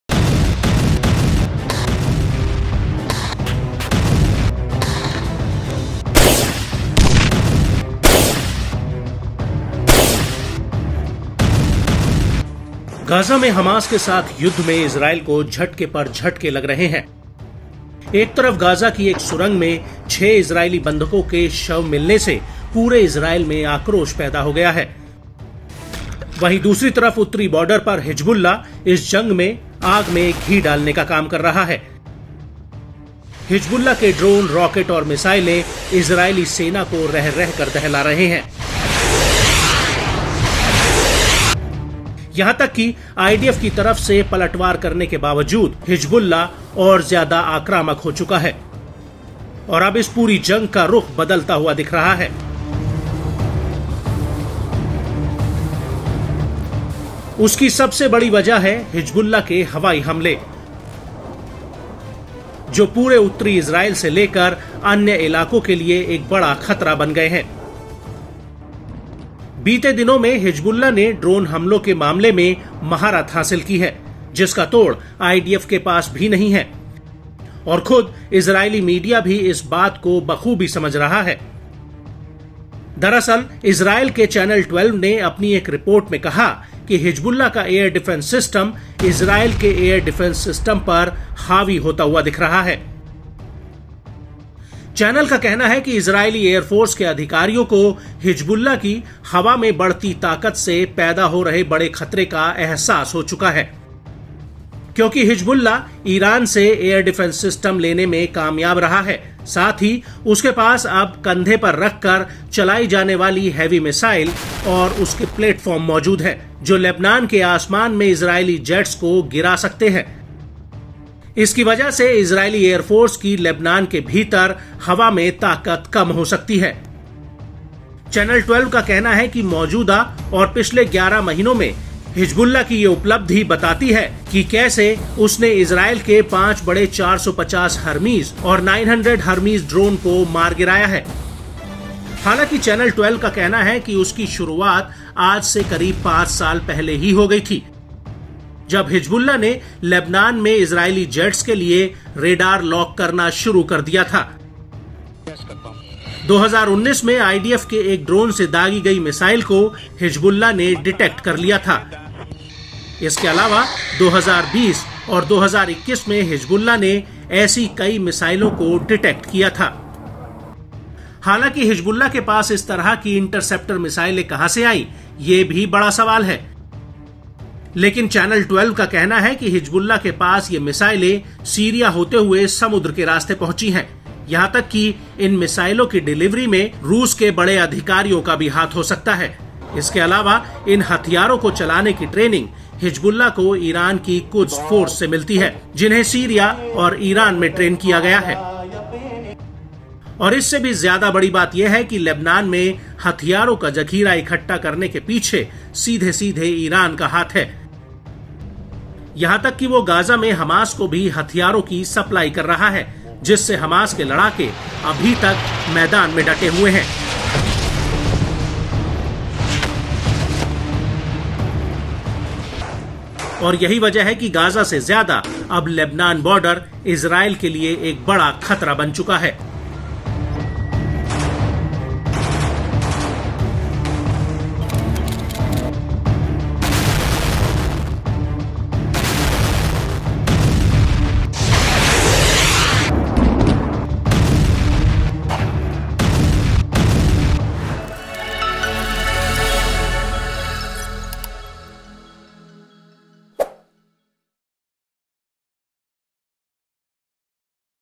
Israel Hamas War : हिजबुल्लाह की आसमान में बढ़ती ताकत से बेचैन हुआ इज़रायल ! | N18G| Gaza | Rafah
Amidst the Israel Hamas War, the Hezbollah Air Attack has posed a major threat in Northern Israel. In the recent past, Hezbollah has mastered the matter of Drone Attack, which the IDF does not have an answer to. The Israeli media itself is understanding this very well. In fact, Israeli Channel 12 has said in one of its reports that the Hezbollah Air Defense System is becoming dominant over the Israel Air Defense System.

Israel Hamas War के बीच Hezbollah Air Attack से Northern Israel में बड़ा खतरा पैदा हो गया है. बीते दिनों में Hezbollah ने Drone Attack के मामले में महारत हासिल की है, जिसका तोड़ IDF के पास नहीं है. खुद इज़रायली मीडिया भी इस बात को बखूबी समझ रहा है.. दरअसल, Israeli Channel 12 ने अपनी एक रिपोर्ट में कहा है कि Hezbollah Air Defense System Israel Air Defense System पर हावी होता जा रहा है.